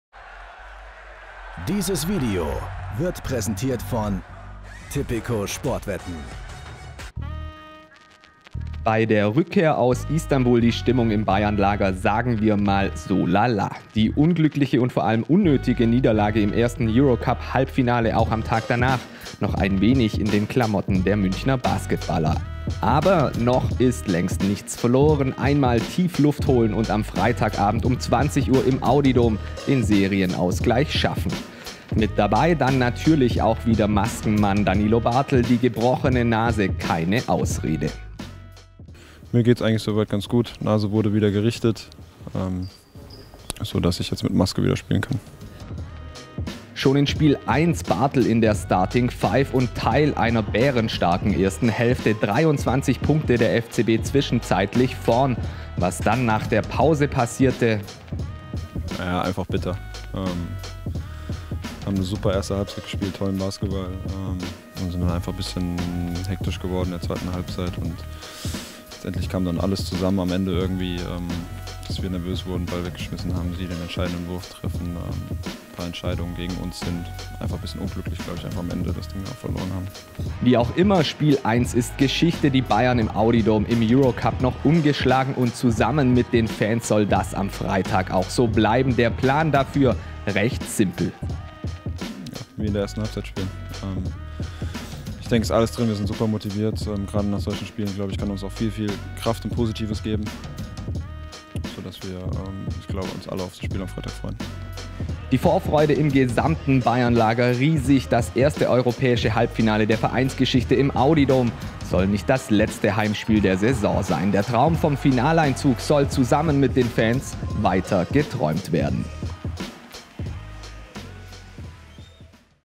EC-Halbfinale 2: Die Bayern wollen drittes Spiel erzwingen
Nach der bitteren Niederlage im ersten Halbfinale blicken die Bayern kämpferisch auf das zweite Spiel im Audi Dome am Freitag um 20 Uhr.